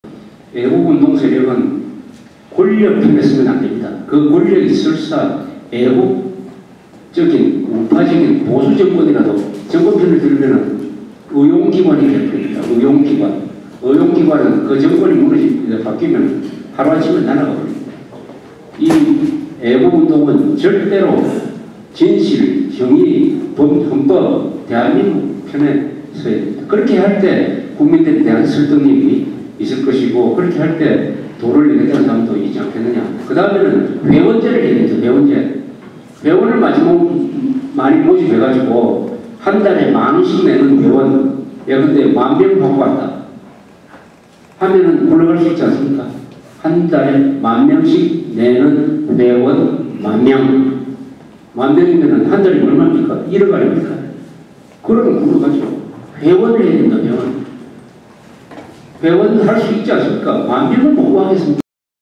[조갑제 자국본 7-1] "애국운동세력, 권력 편에 서면 어용기관 돼"
자유대한지키기 국민운동본부.  2009년 9월25일 오전 영락교회 선교관.